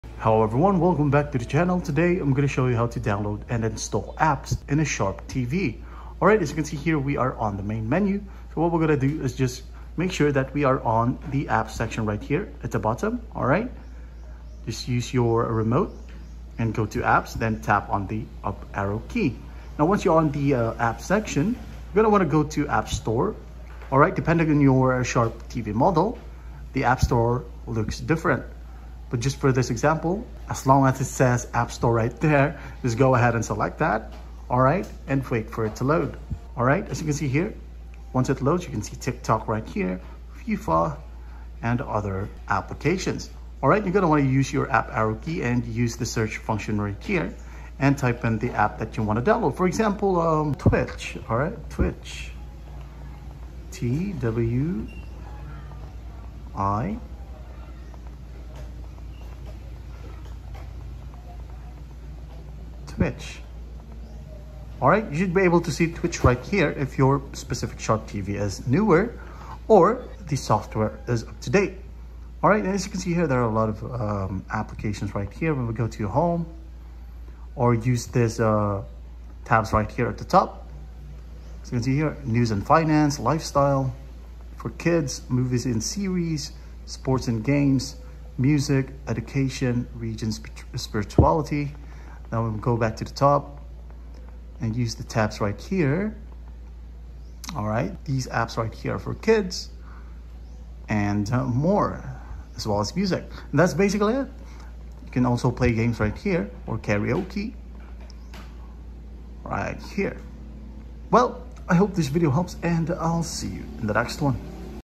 How To Download & Install Apps On Sharp Smart TV
Welcome, in this video I will show you How To Download & Install Apps On Sharp Smart TV.